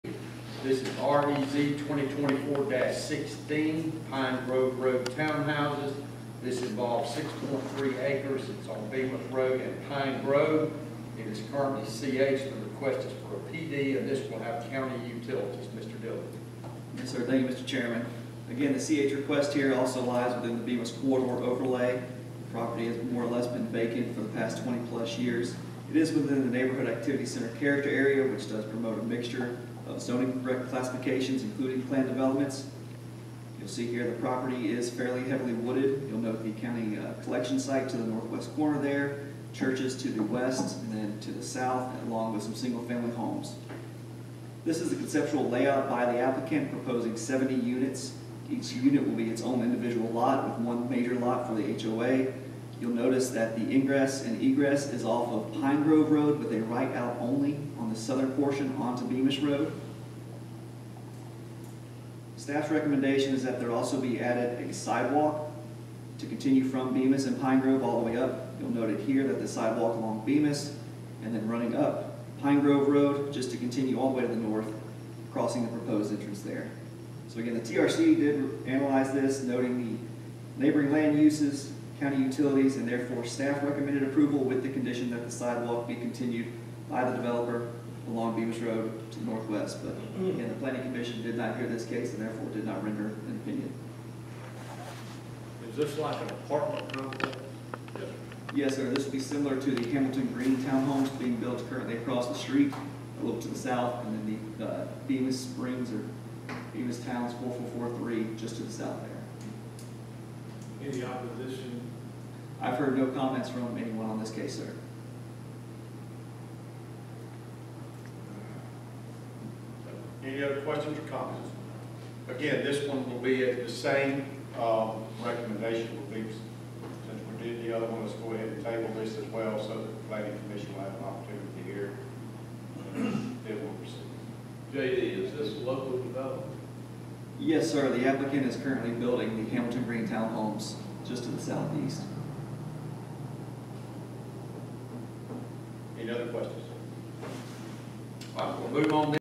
5b. REZ-2024-16 Pine Grove Road Townhouses, 6.3ac, C-H to P-D, @ LCC Work 2024-09-09
5b. REZ-2024-16 Pine Grove Road Townhouses, 6.3ac, Bemiss Rd & Pine Grove, C-H to P-D, County Utilities

This was also not heard by GLPC and same tabling question.

Commissioner Wisenbaker asked if it was a local builder and Planner JD Dillard side stepped the question by answering that it was the same builder that was building the town homes on Bemiss just to the south.